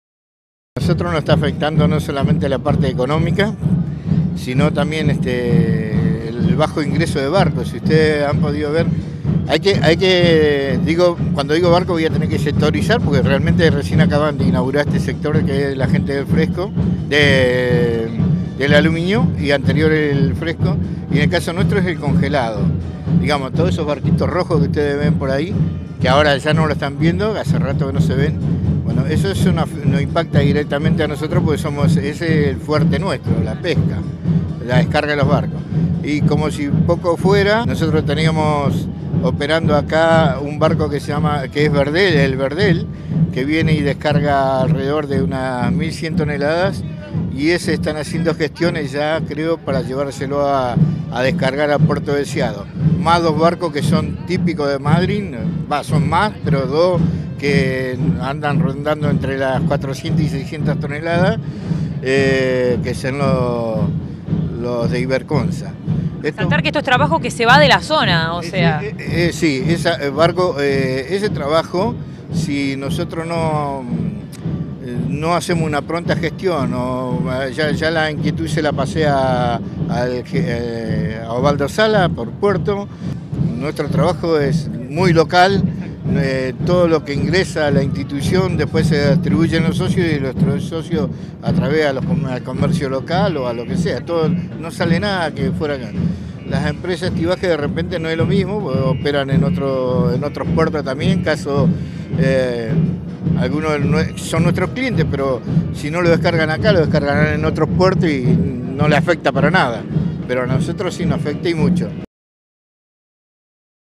Estibajes Madryn: "Preocupados por bajo ingreso de barcos"
Pesca: Crisis de congeladores.
"Barco de 1100 Tn se va a Puerto Deseado".
Nota: Enrique Goyeneche, Estibajes Madryn.